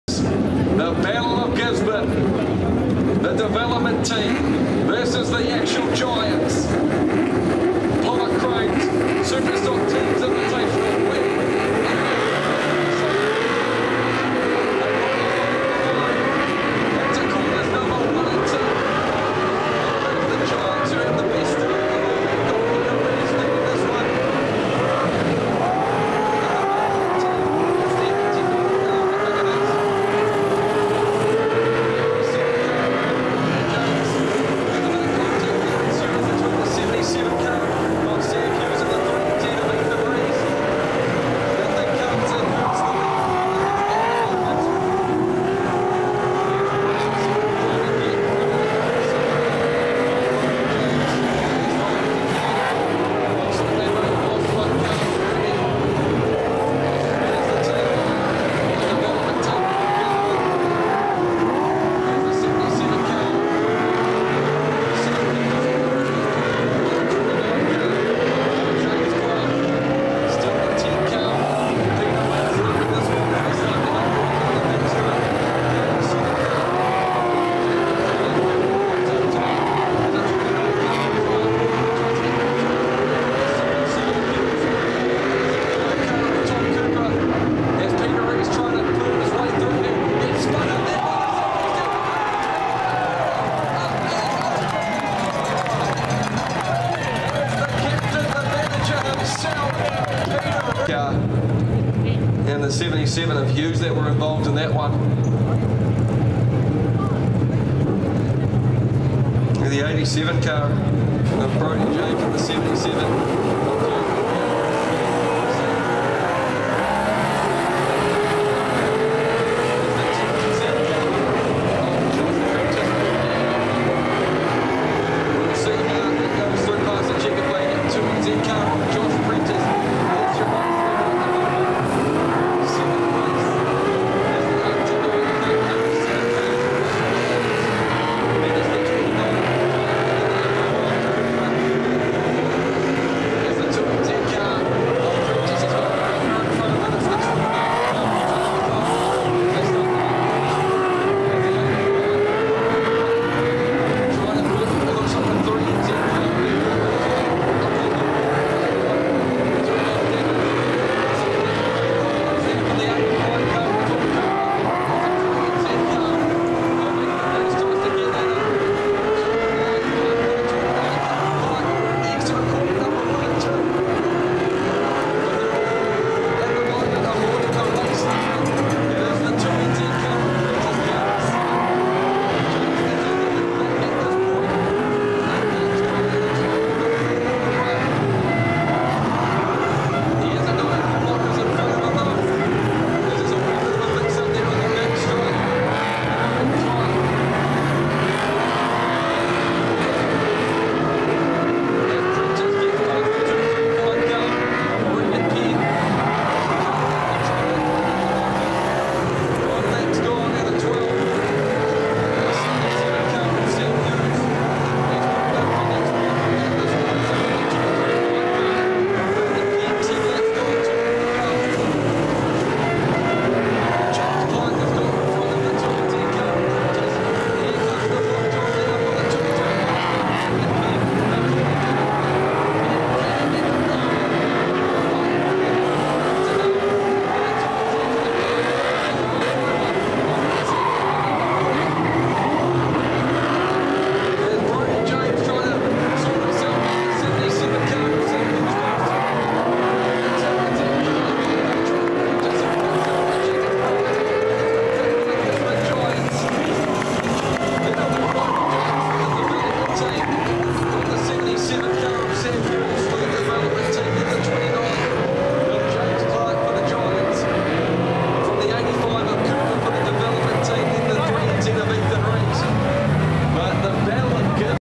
Gisborne Giants vs Gisborne Development Semi Final 2 Huntly 6 May 2023
After multiple rainoffs / postponements Huntly Speedway ran the Superstock Teams Invitational as their last meeting of 2023 Season